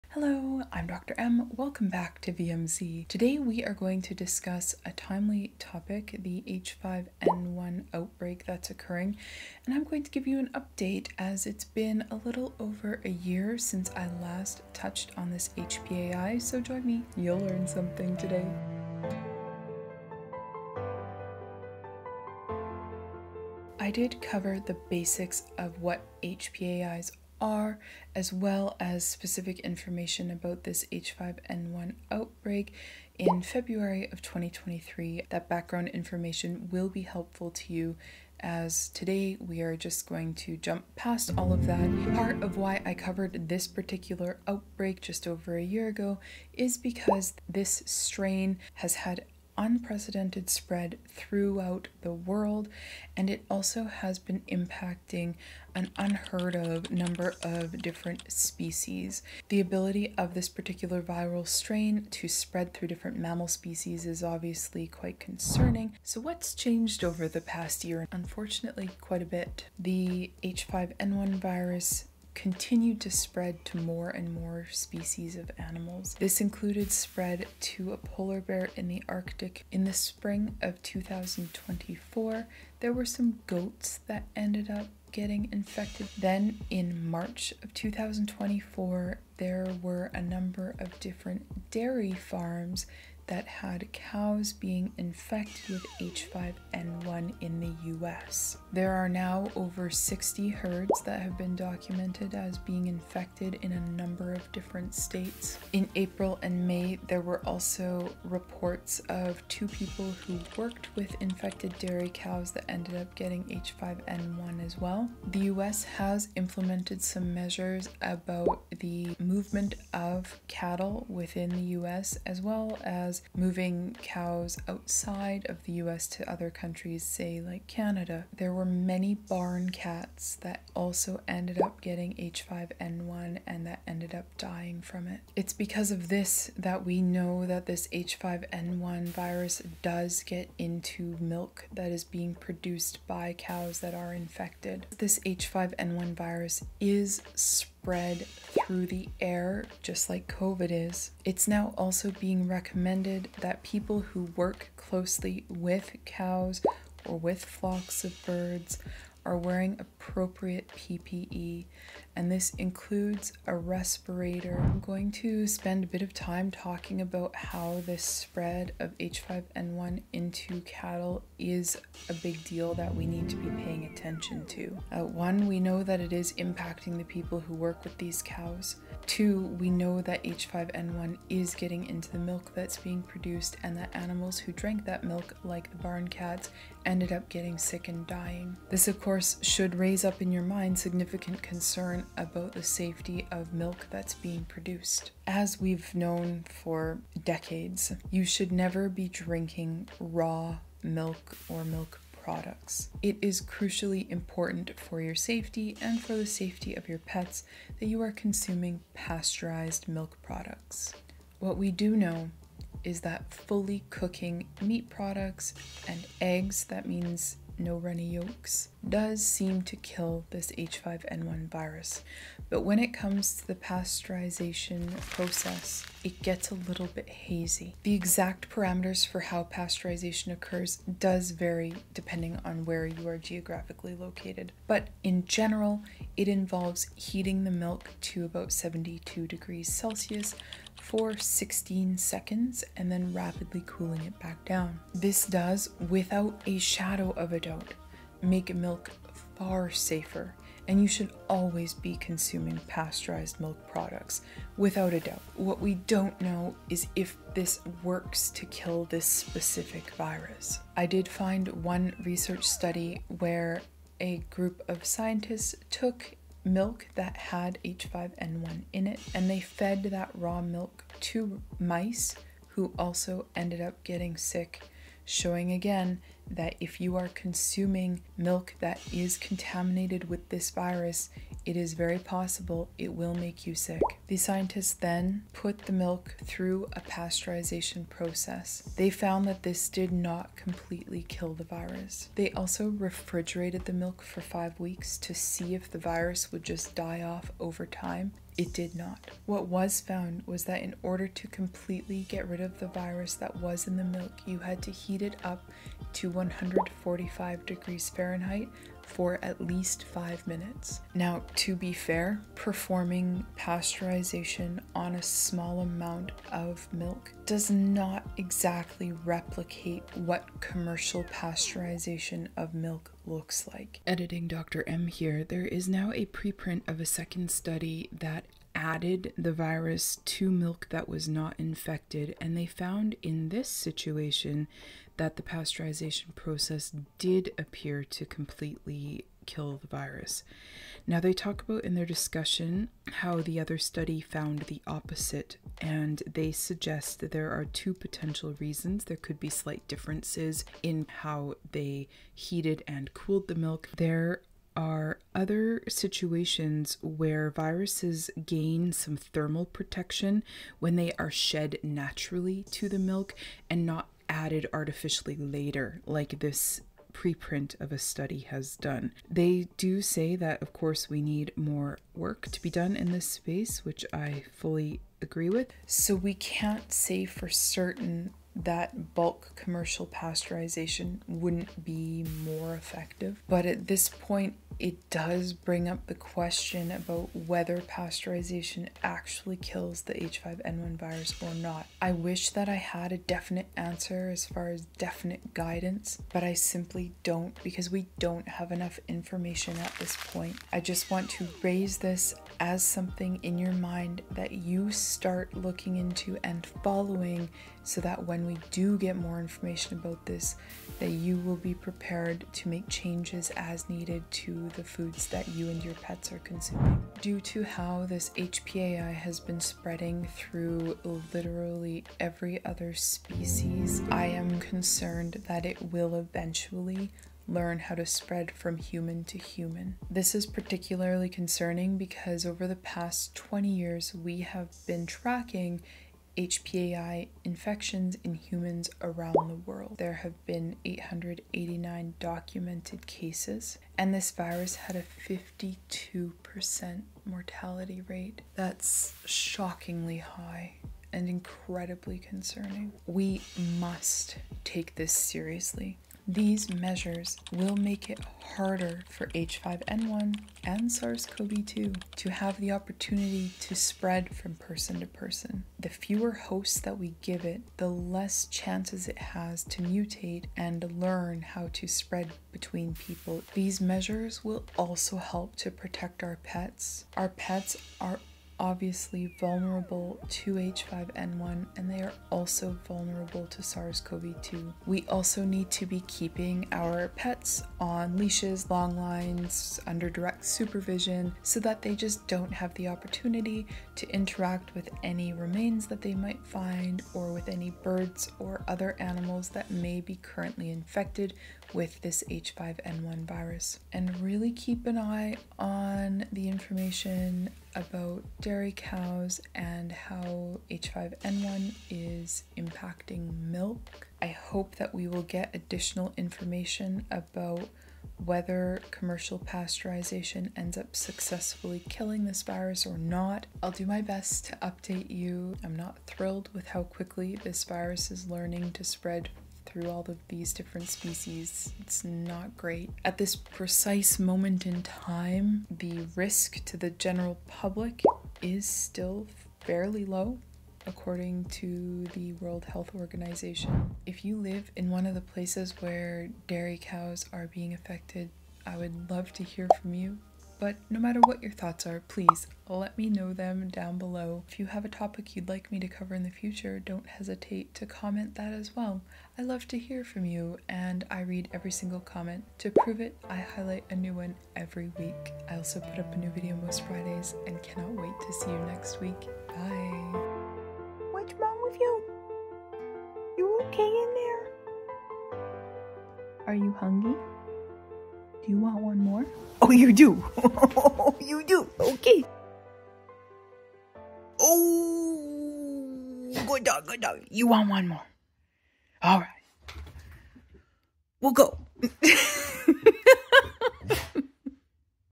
H5N1 Outbreak Update | Veterinarian Explains
Today's video covers an update about what you need to know regarding the current highly pathogenic avian influenza H5N1 outbreak. Unfortunately, this virus is now spreading in ways that are raising additional concerns about the food chain and the update isn't great.

Please comment with your ideas for future video topics!